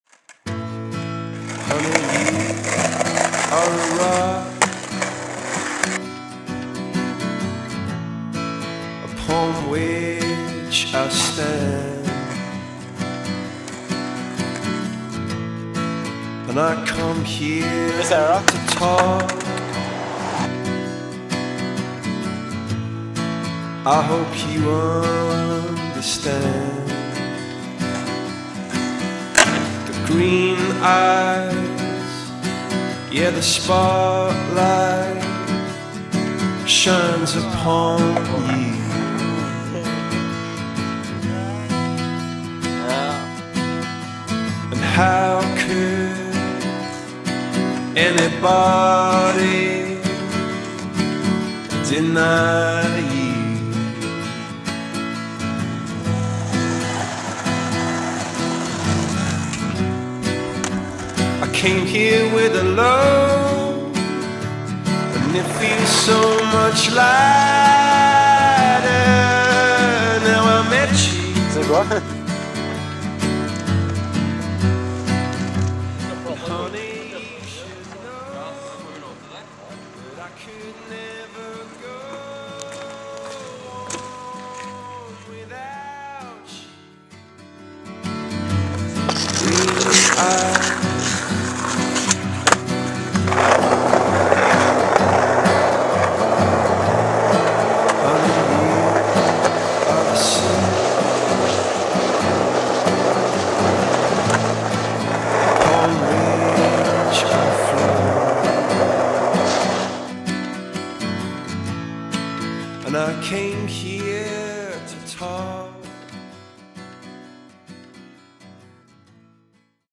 Winter Throw-Away Footage
Just mainly boring footage we'll never use or anything. It's cold here up in Norway and spirit is low.
Snowing too, gotta get something before asphalt is gone
(maybe)